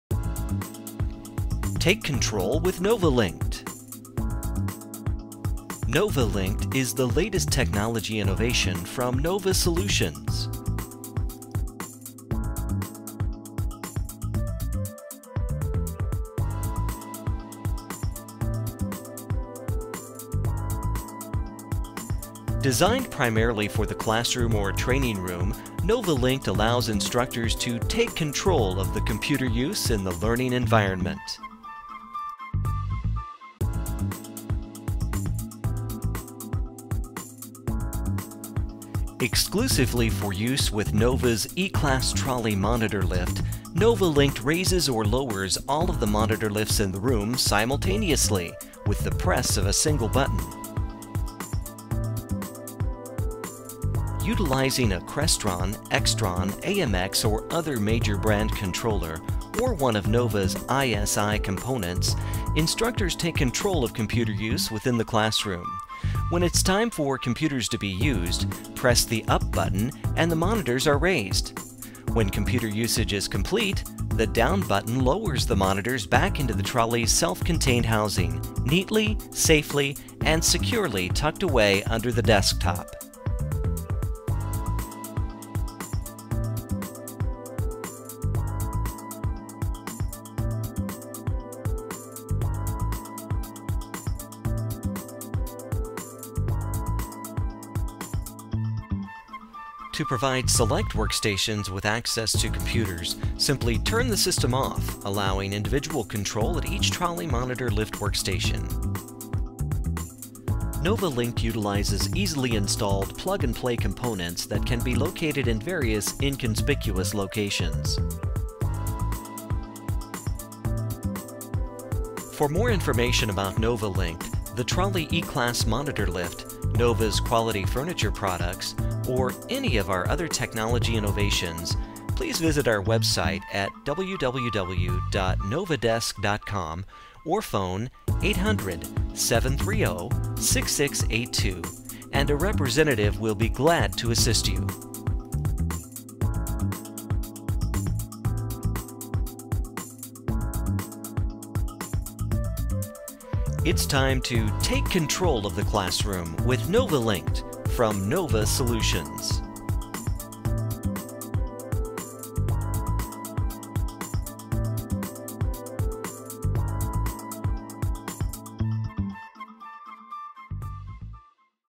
NOVALinked (Narrated)
Learn all about NOVA's innovative solution for controlling computer access in the learning environment.  Designed for classrooms and training rooms, NOVALInked™ provides instructors control of The Trolley™ E-Class monitor lifts simultaneously.